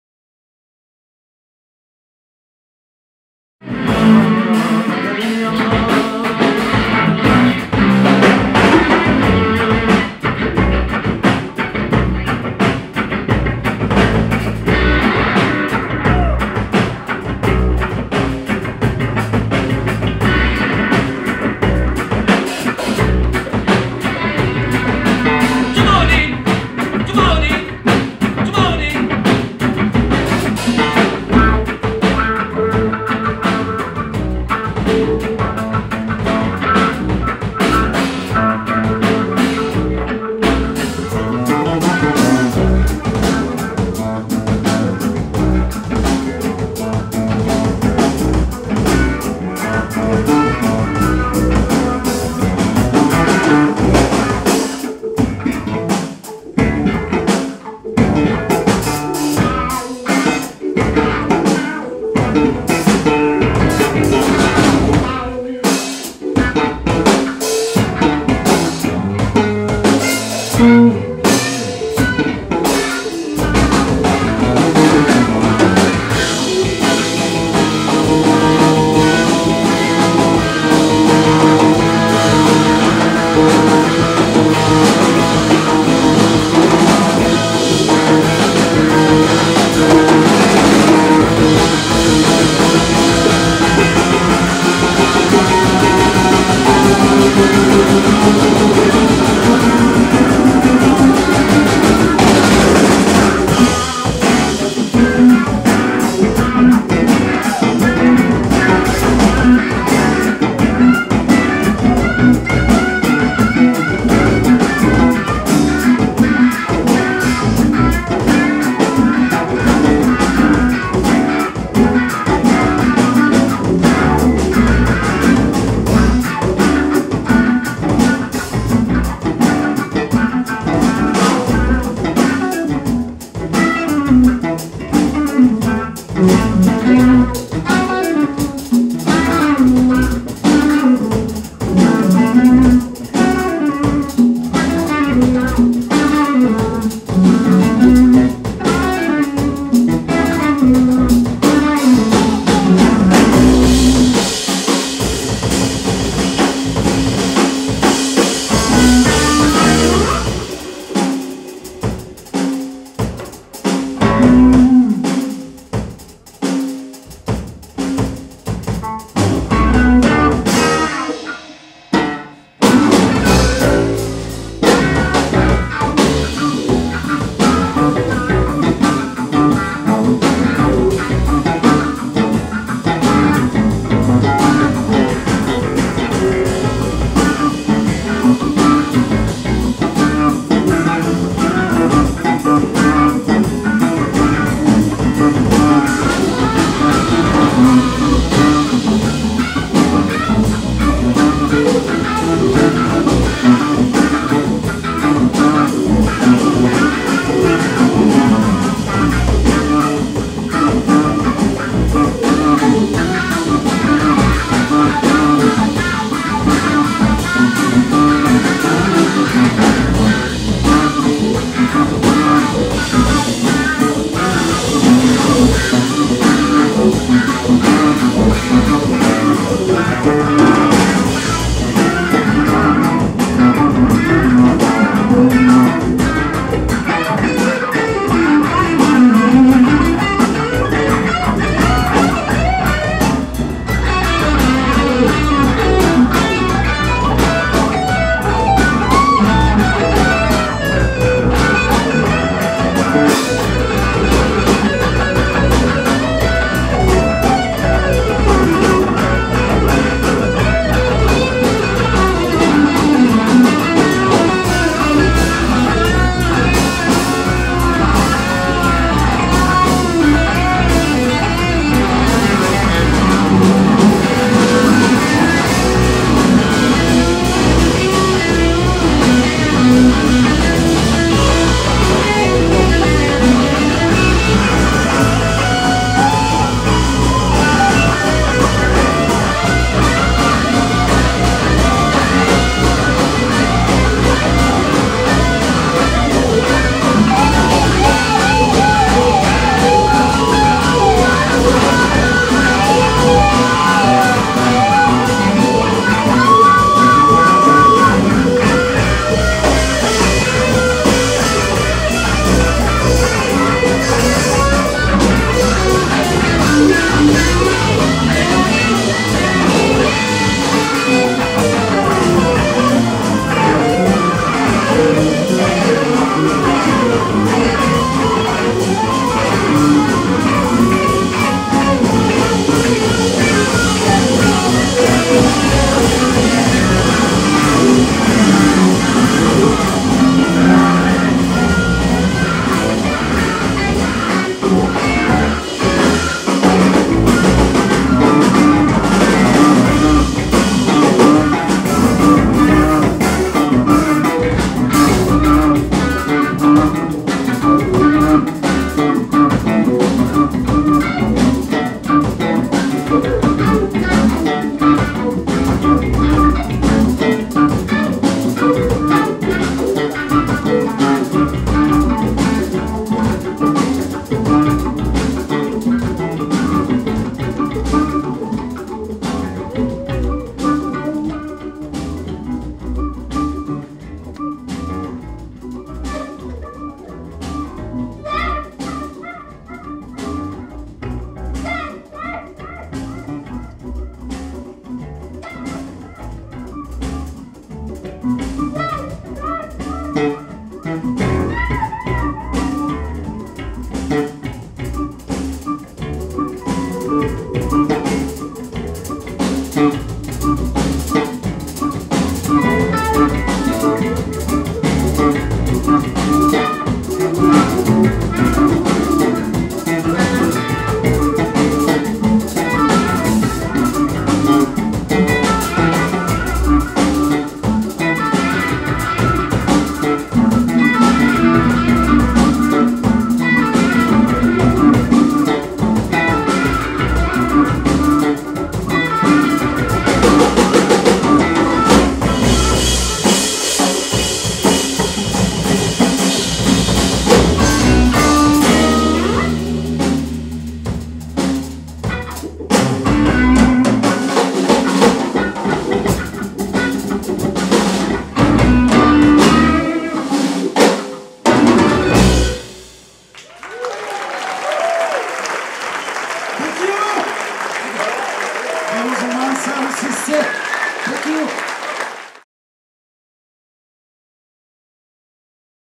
1/Noonan, Bajakian, Ribot, Tacuma, Diabate
Noonan (drums), Bajakian(guitar), Ribot(guitar) Tacuma(bass),Diabate(congas )  If you do not see a High Quality choice just below the screen, substitute this URL :